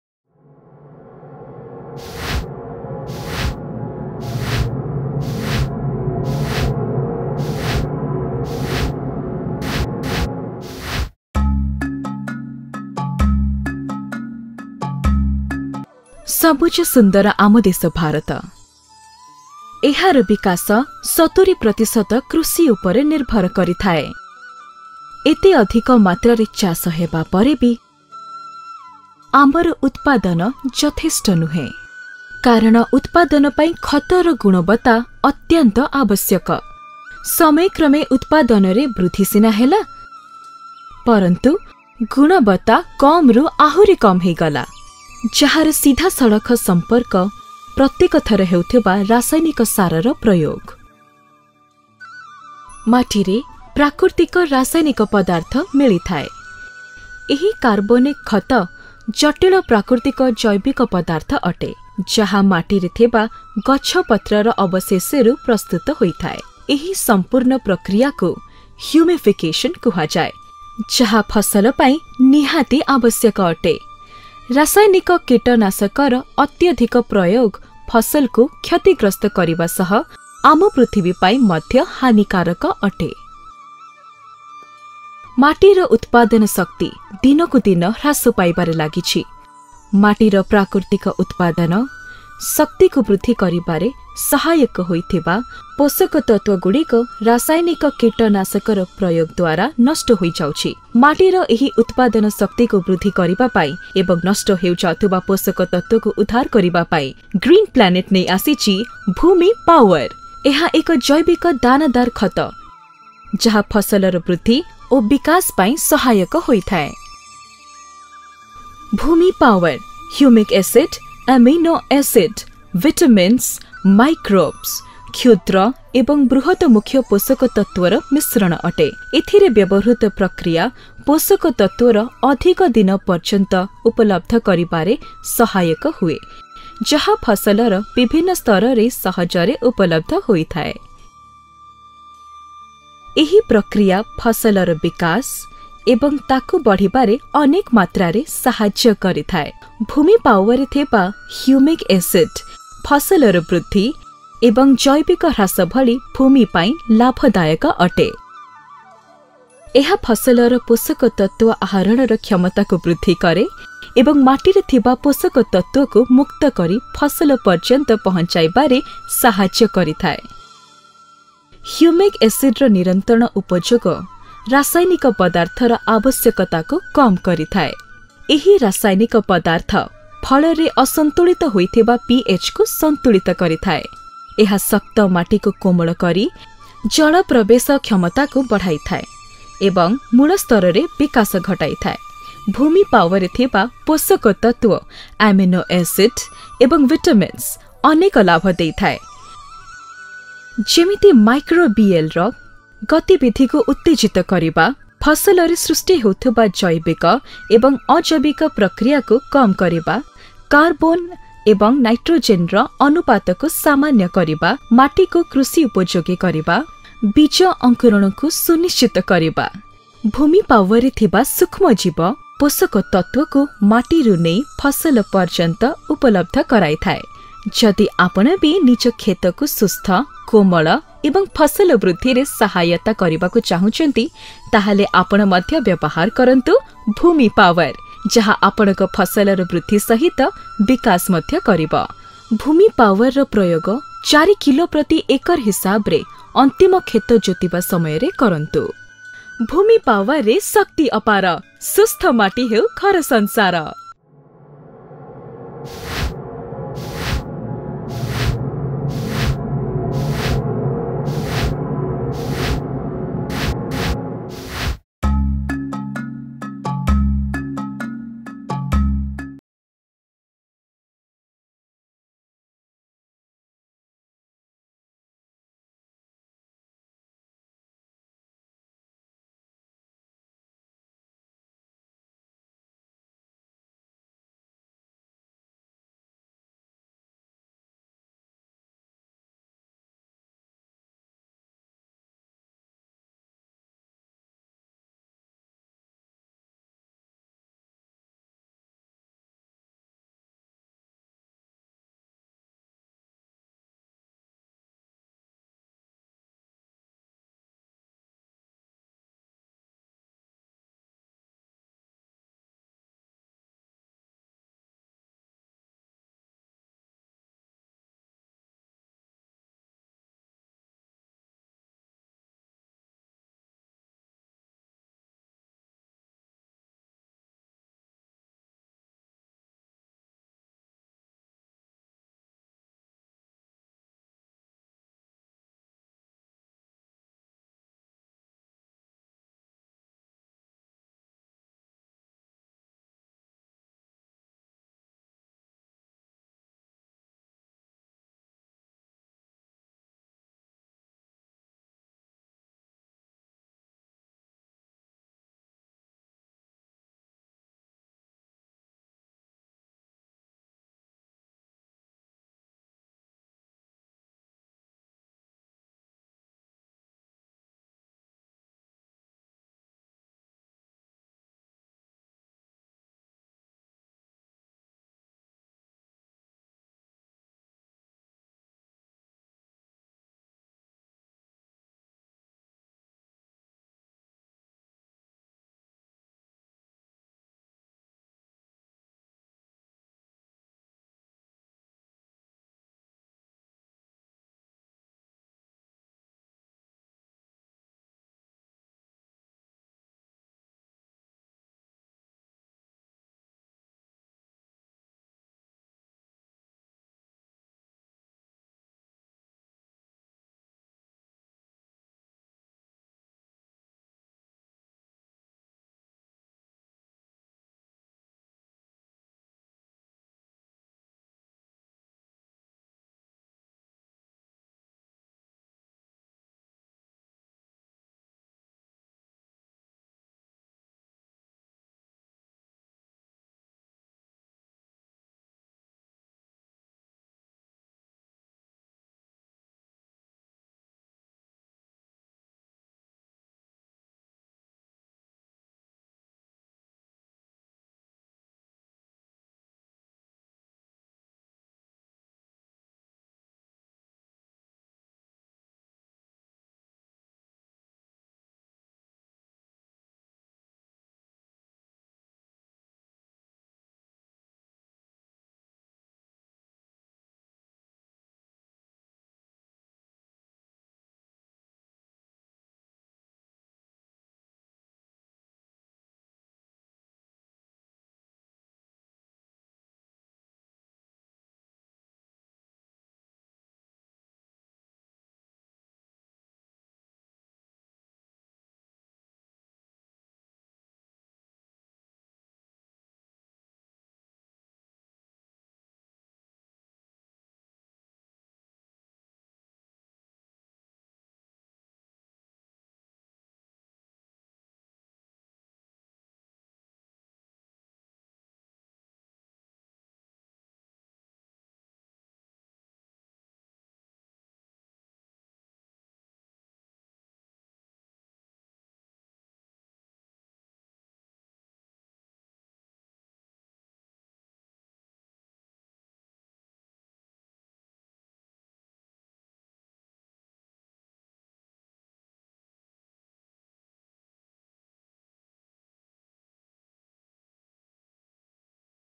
Green Planet Bhoomi Power - Oriya
Bhoomi Power is an organic granular formulation with long lasting technology for enhancing growth and development of the plant. It contains humic acid, vitamins, amino acids and seaweed.
n the previous years, the soil was known as humus as whole but now days the compost and the organic matter we added in the soil is called humus. The humus present in the soil, help the plants to grow and develop successfully but with the increased usage of chemical fertilizers the level is going down.
 These humus compounds are complex natural organic substances that are formed in the soil from plant residues by the process of humification. The process of humification is affected because of the heavy usage of chemical fertilizers.
 Bhoomi Power contains humic acid because of its beneficial impact on growth and cultivation of crop and particularly in organically deficient soils. Humic acid is not a fertilizer but it is a compliment to fertilizer.